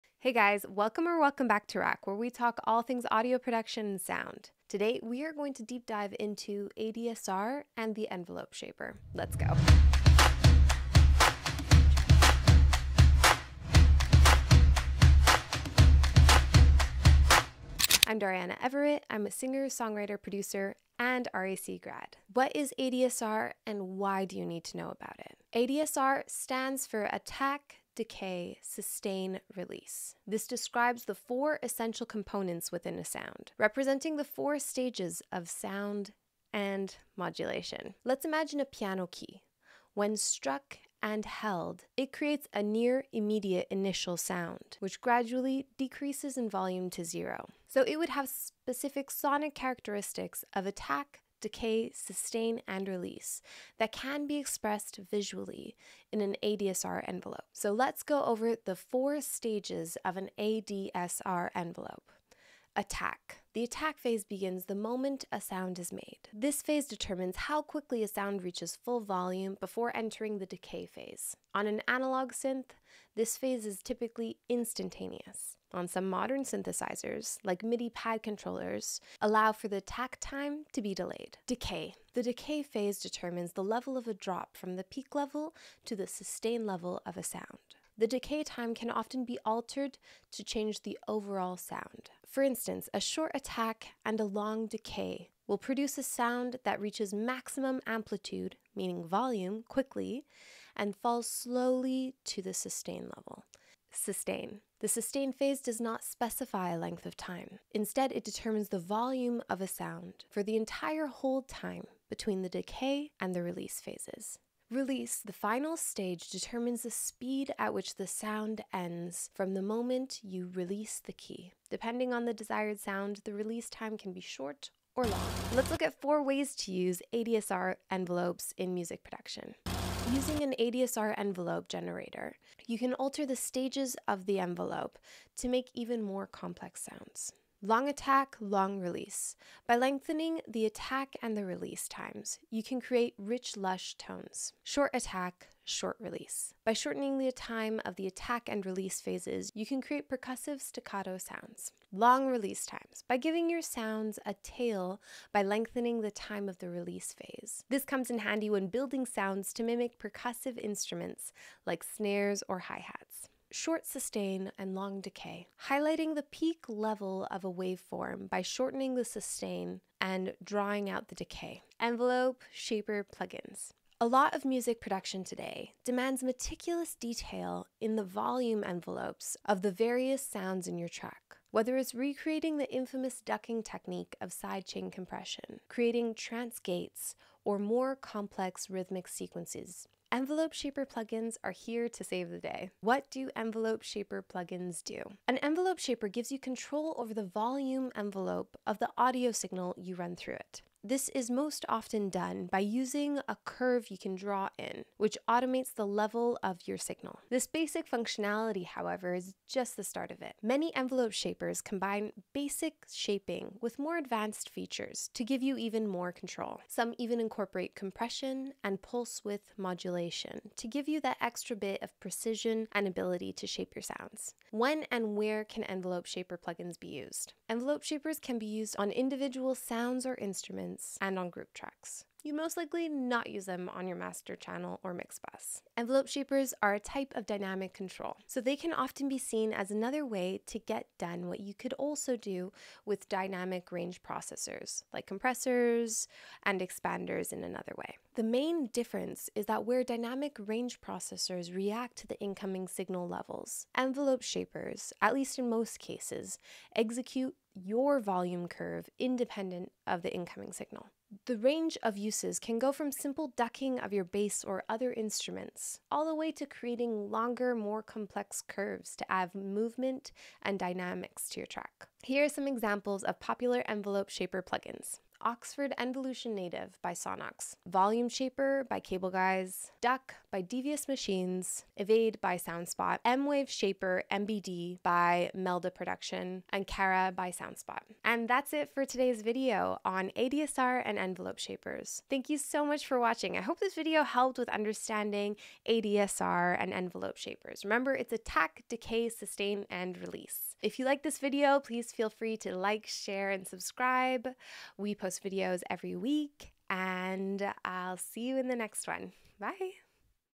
ADSR | Envelope Shaper Plugin
Have you ever wondered what ADSR is?
Attack
Decay
Sustain
Release

In today's video, @DariannaEverett breaks down the mystical reality of ADSR and envelope shapers & when and how to use them.

Become the artist/ music producer you always wanted now in 2023.

Recording Arts  Canada